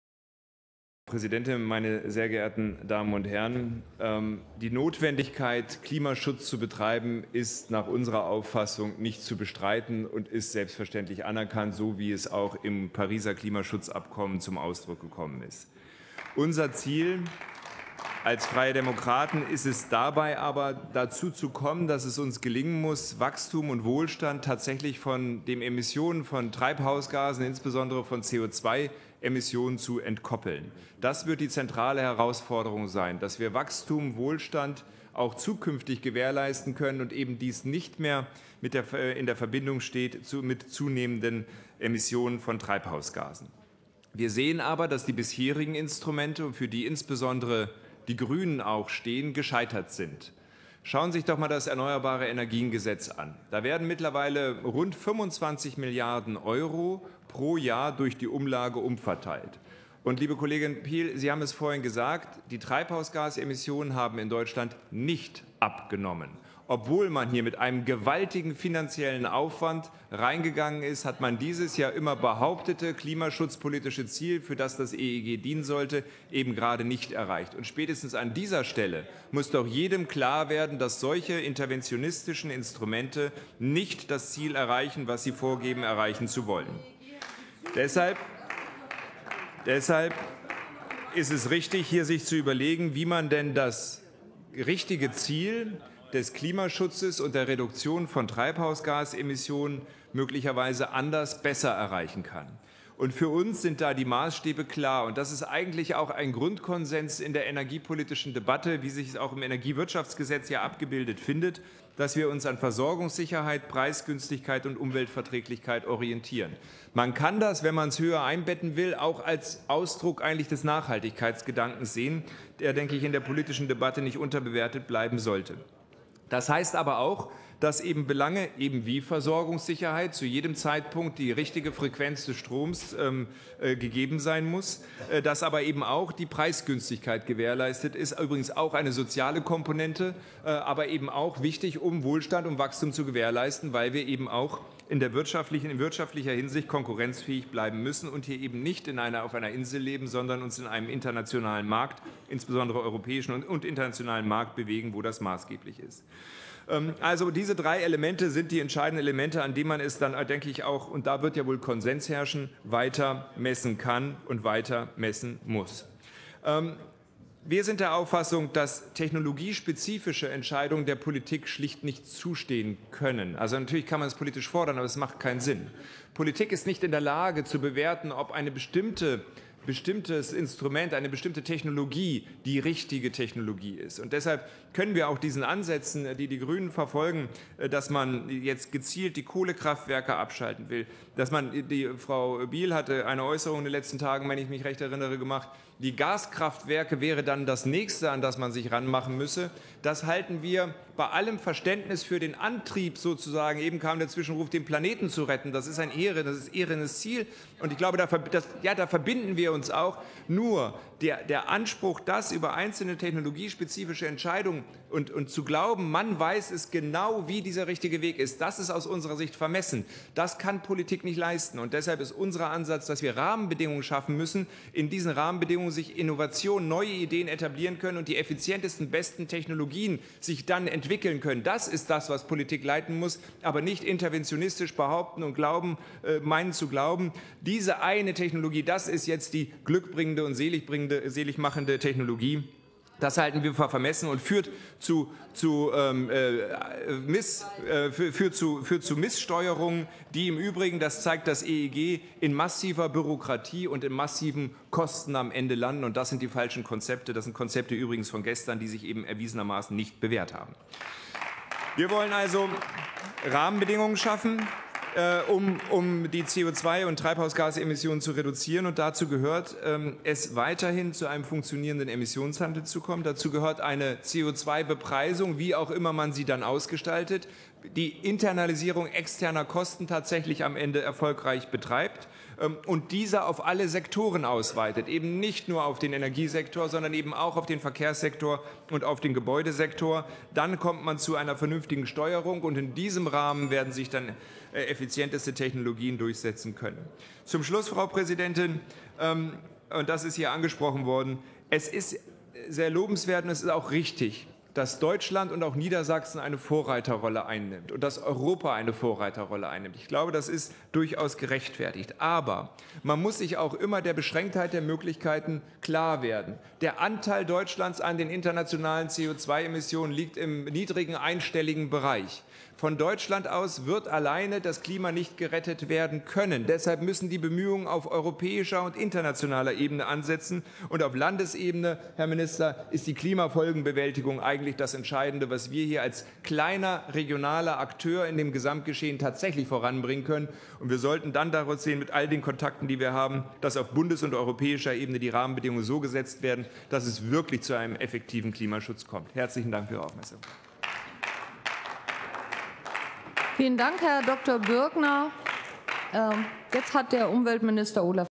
Stefan Birkner zur Aktuellen Stunde zum Thema Klimaschutz
Aktuelle Stunde: Kein Kohleausstieg, kein Geld, kein Plan - die Landesregierung versagt beim Klimaschutz